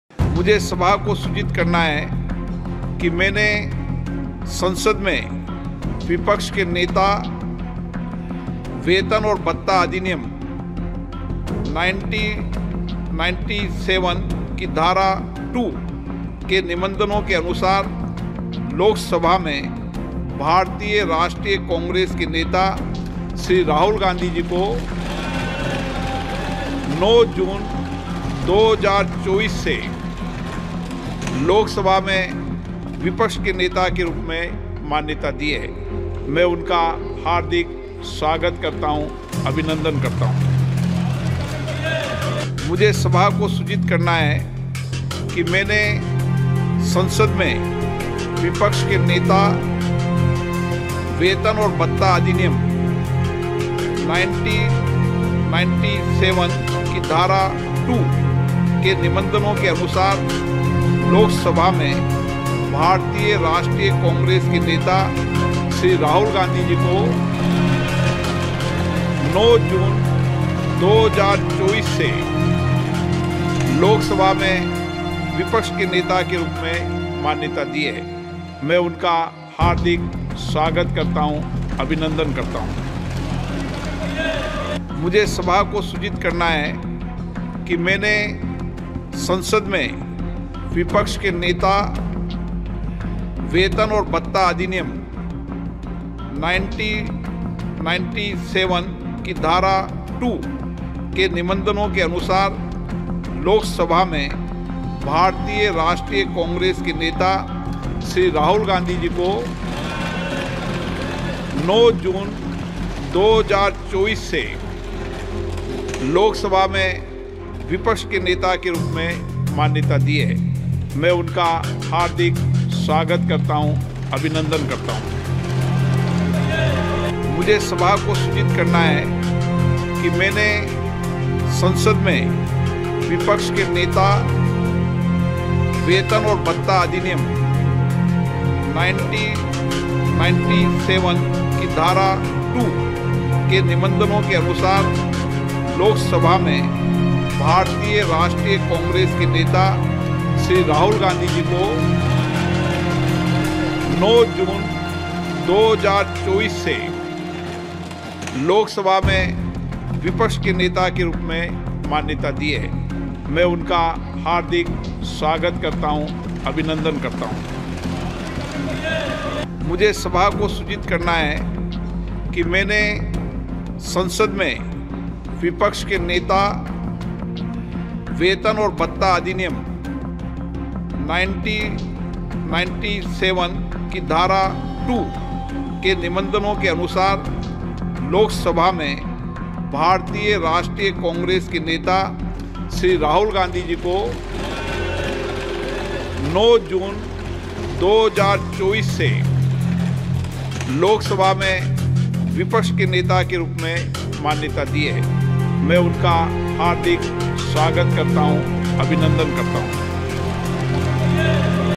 Rahul Gandhi ने Om Birla को Loksabha Speaker चुने जाने पर दी बधाई, बोले- विपक्ष की आवाज को भी...
माननीय राहुल गांधी ने ओम बिरला को लोकसभा के स्पीकर चुने जाने पर बधाई दी। उन्होंने कहा कि विपक्ष की आवाज को भी समझा जाना चाहिए।
Congratulations to Om Birla on being elected as the Speaker of the Lok Sabha, Rahul Gandhi emphasized understanding the voice of the opposition